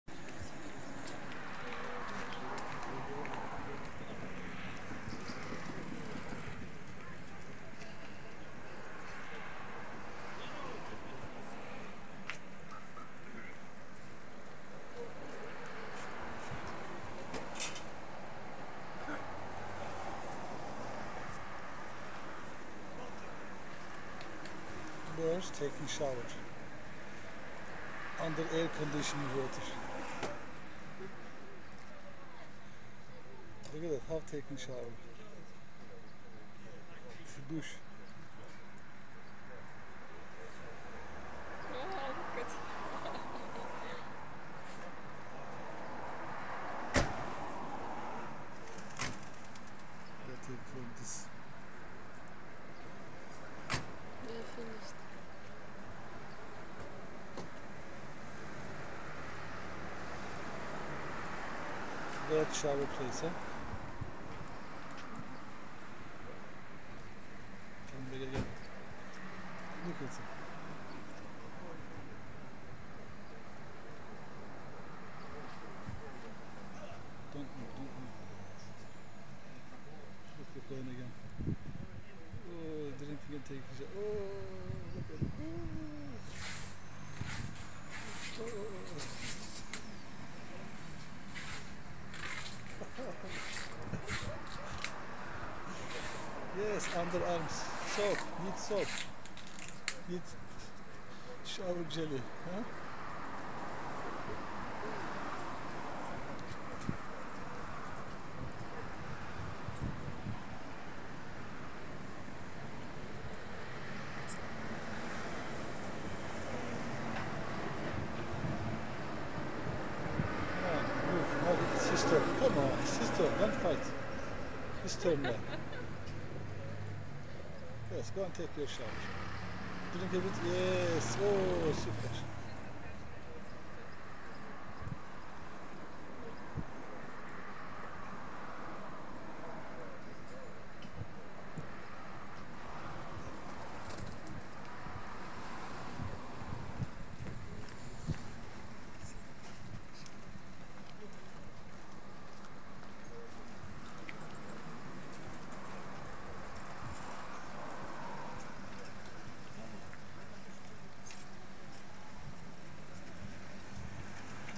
Pigeon taking a shower.
By luck I have recorded these sweet pigeons taking shower under the dripping water of an air conditioning on a very hot day.

Sicak bir yaz gunu klimanin damlayan suyunun altinda sirayla dus alan guvercinleri tesadufen kaydettik ve goruntuleri paylastik.